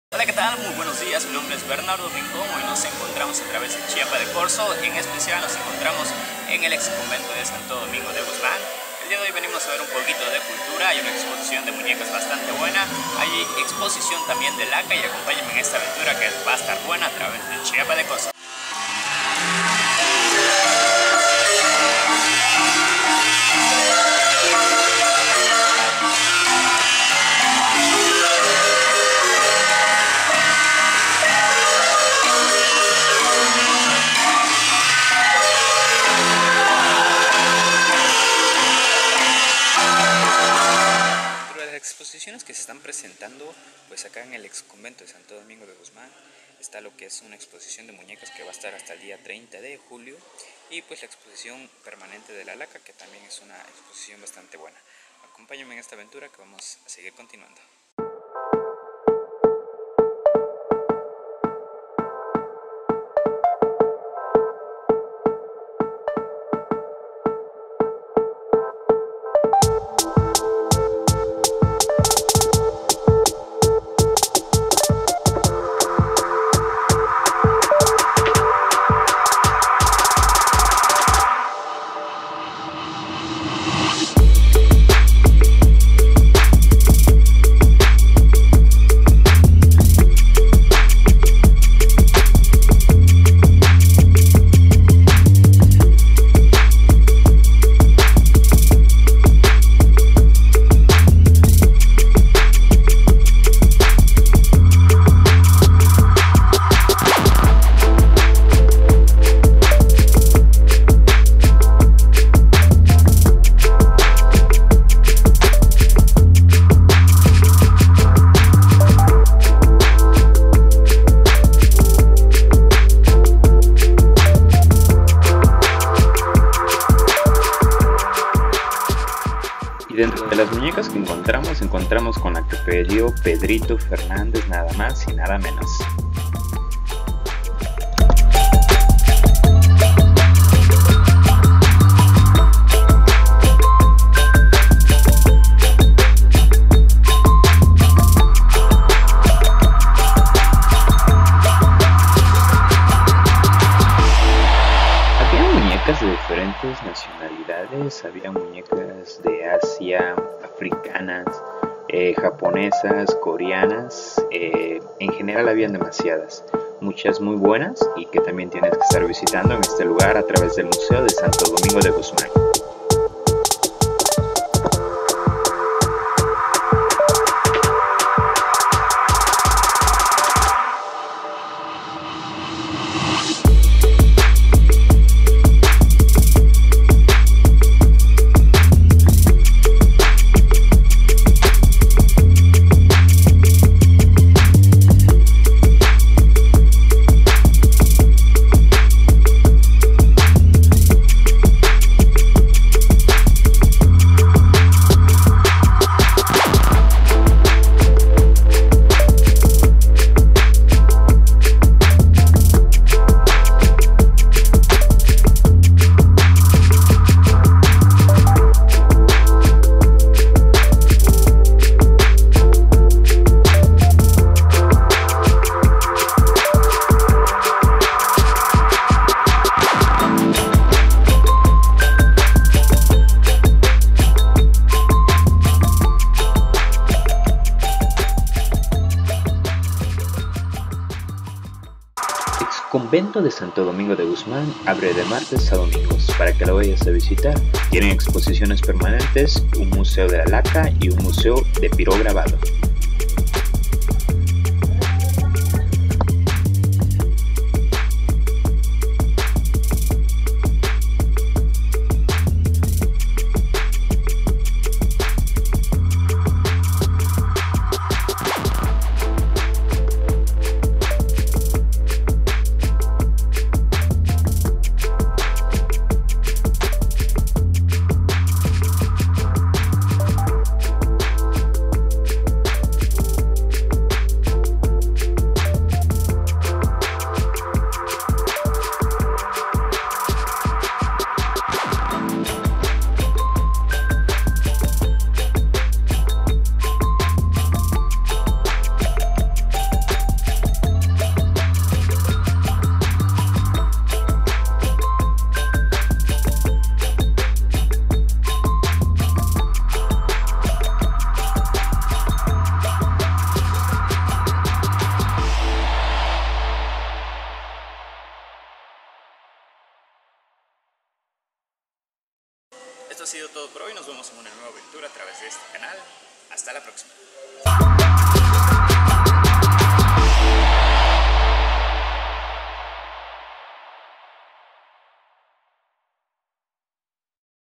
La casa de las muñecas en Chiapa de Corzo, chiapas. México.
VISITAMOS LA EXPOSICIÓN DE MUÑECAS QUE SE ENCUENTRA EN EL EX CONVENTO DE SANTO DOMINGO DE GUZMÁN EN CHIAPA DE CORZO CHIAPAS, VISITALO DE MARTES A DOMINGO HASTA EL 30 DE JULIO entrada gratis!!.